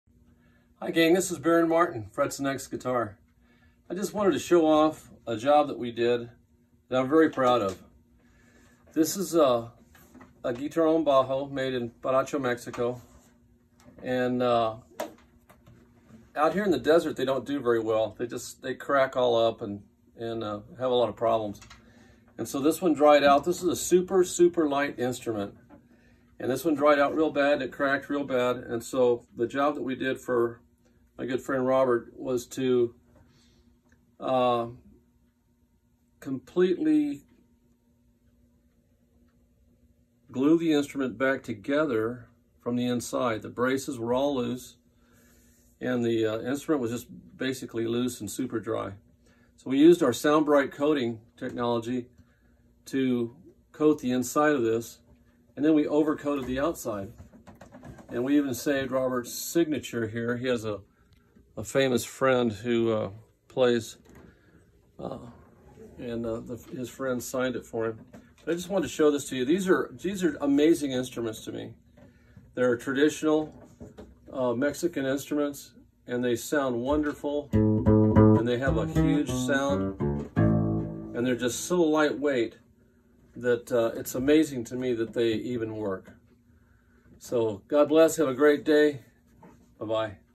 Baron Martin - Guitarron
Frets and necks restores older Guitarron with SoundBright technology.